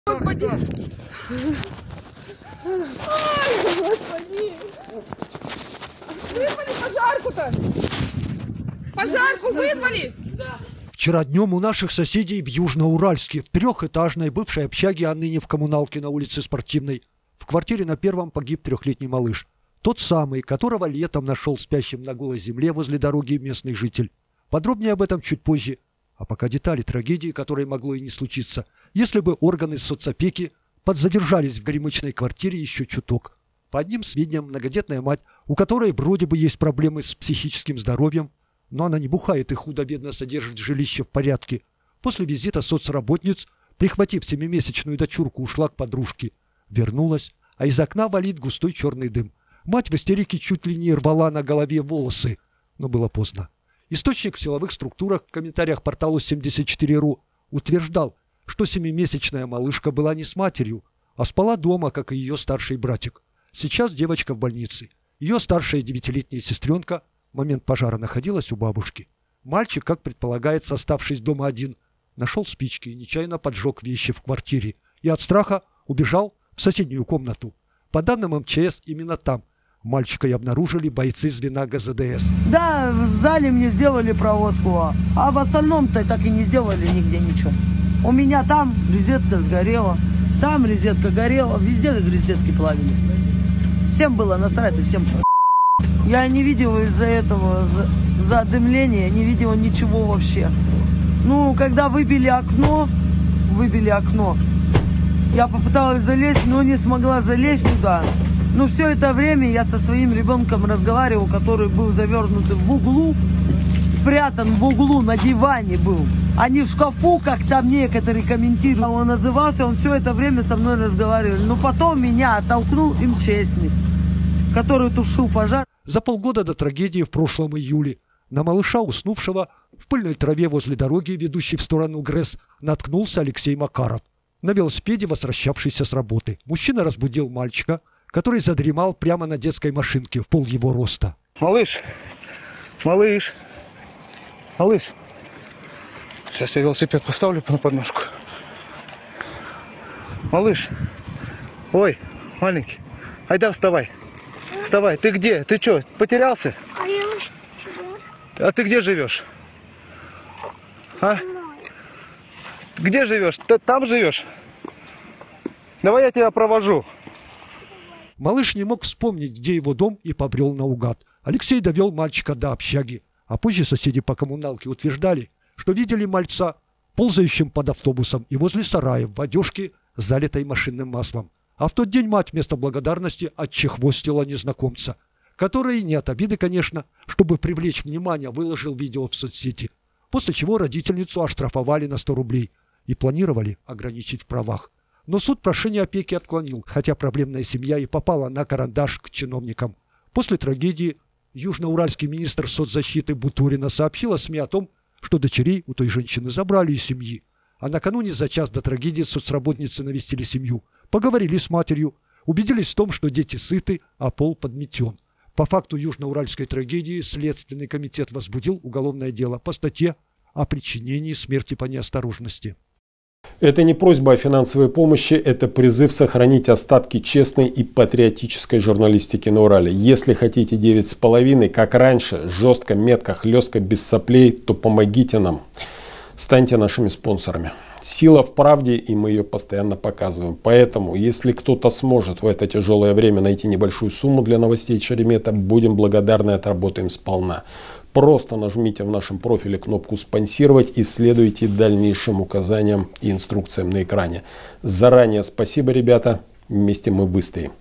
Малыш, спавший при дороге, погиб в пожаре
Многодетная мать после визита соцработниц, ушла к подружке, оставив 3-летнего сына одного дома: мальчик спички нашёл...